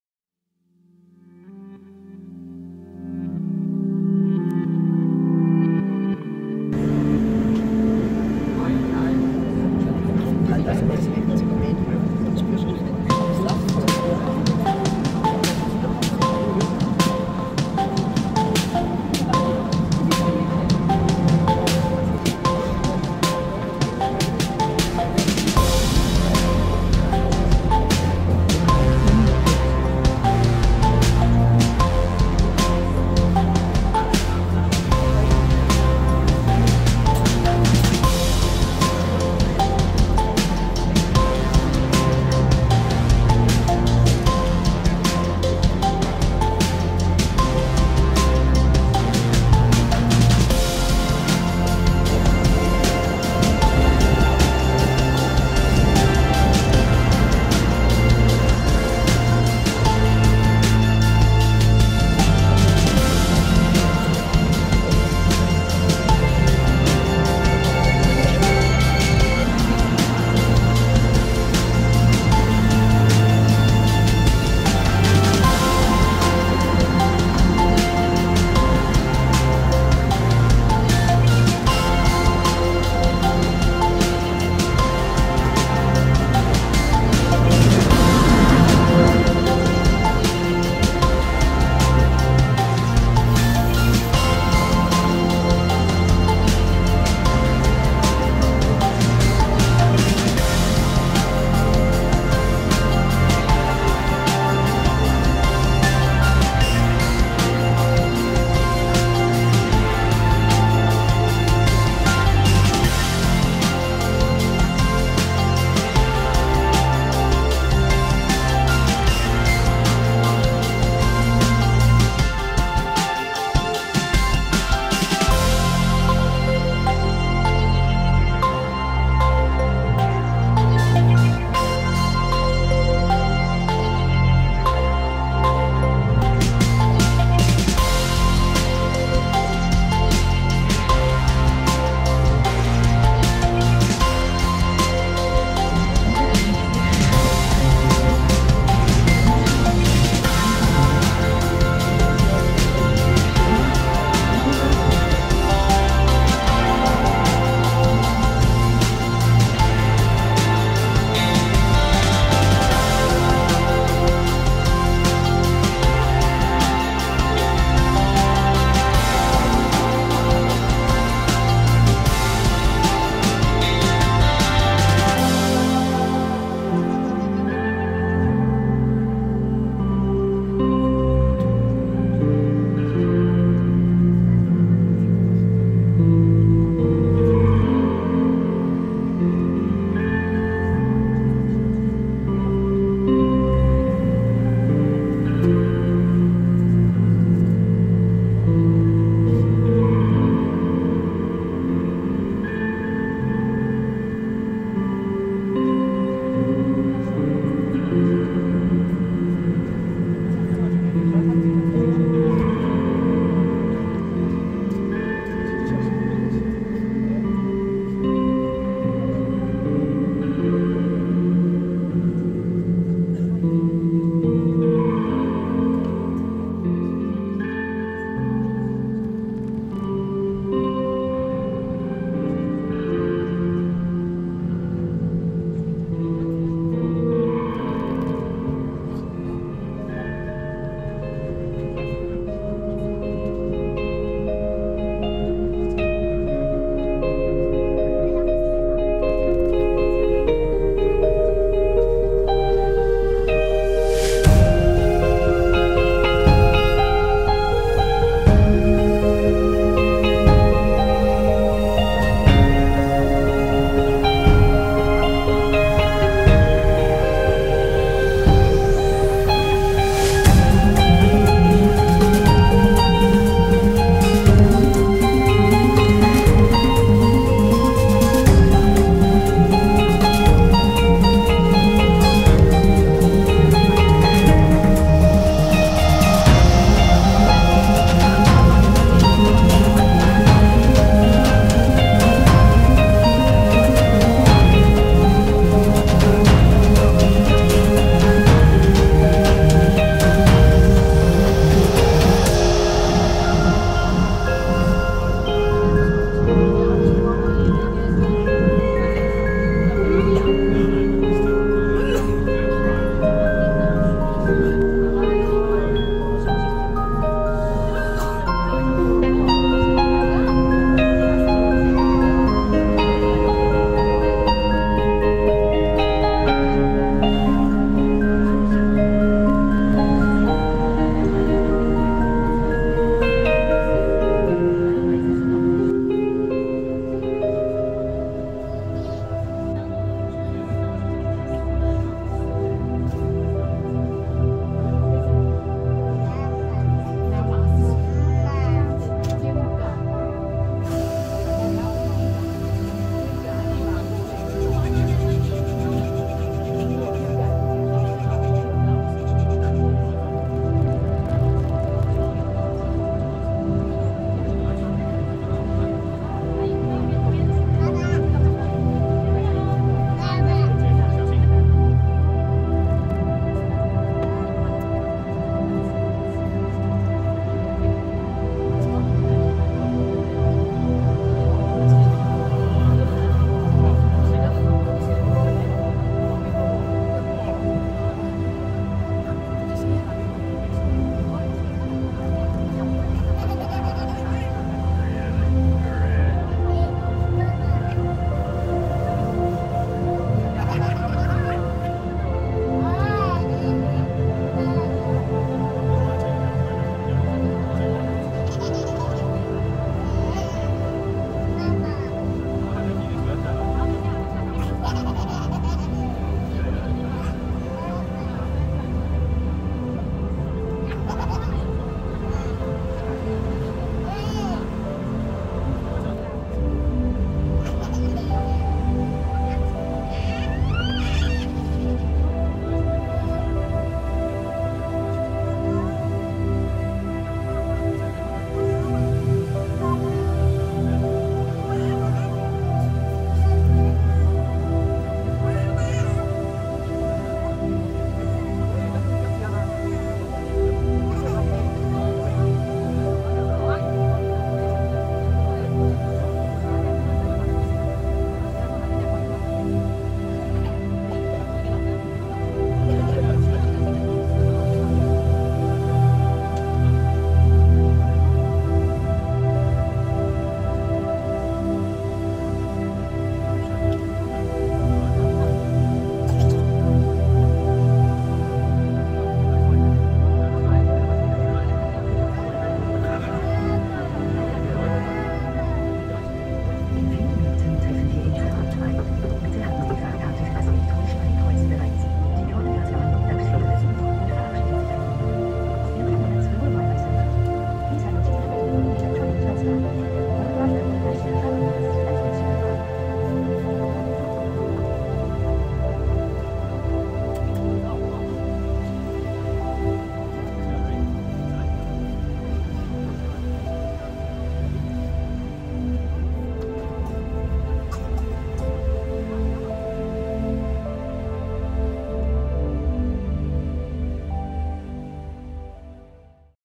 Trainride from Zermatt to Gornergrat
Eine der imposantesten Bahnfahrten ist die Strecke von Zermatt auf den Gornergrat. Während der ganzen Fahrt hat man, wenn das Wetter stimmt, das berühmte Matterhorn in Sicht.
Auf dem Gornergrat erlebt man ein tolles 360-Grad Panorama auf die umliegenden Alpengipfel über 3000 Meter über Meer.
Wichtig: Die besten Plätze in Fahrtrichtung hat man bei der Bergfahrt auf der rechten Seite!

Der schönste Teil der Fahrt sind die letzten 4 Minuten!